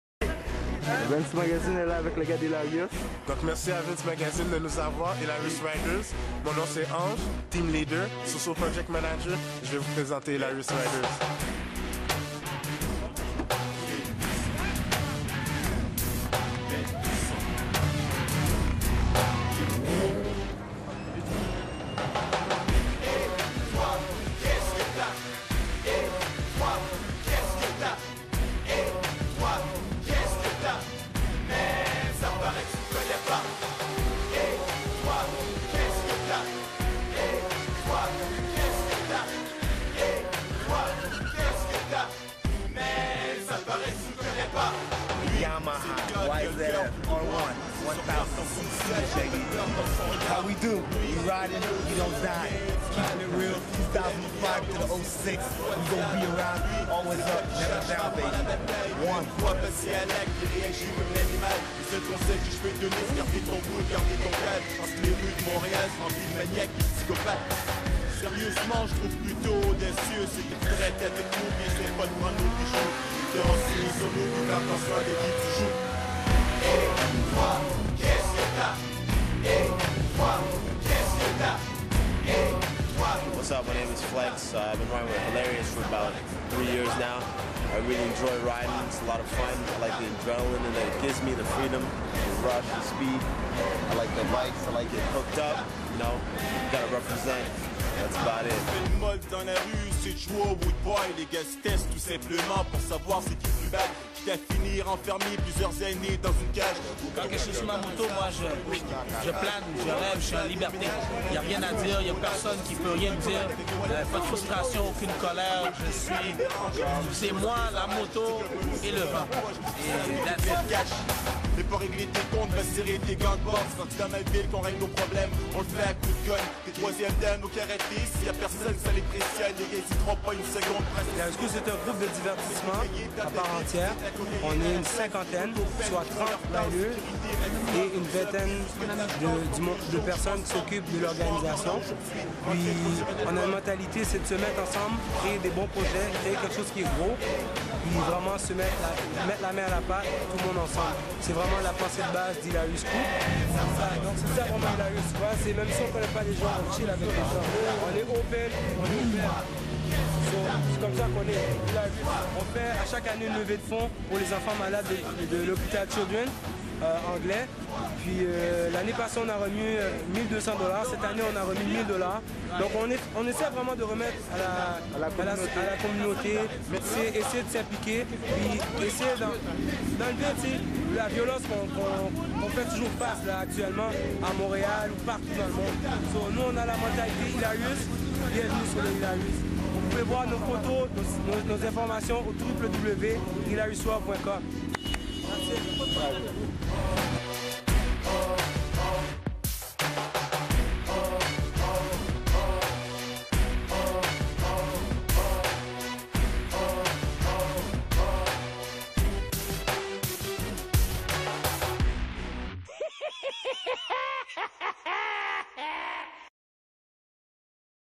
Vens Magazine: Hilarious Riders 2005
Vens Magazine interview of the Hilarious Riders (French)

2005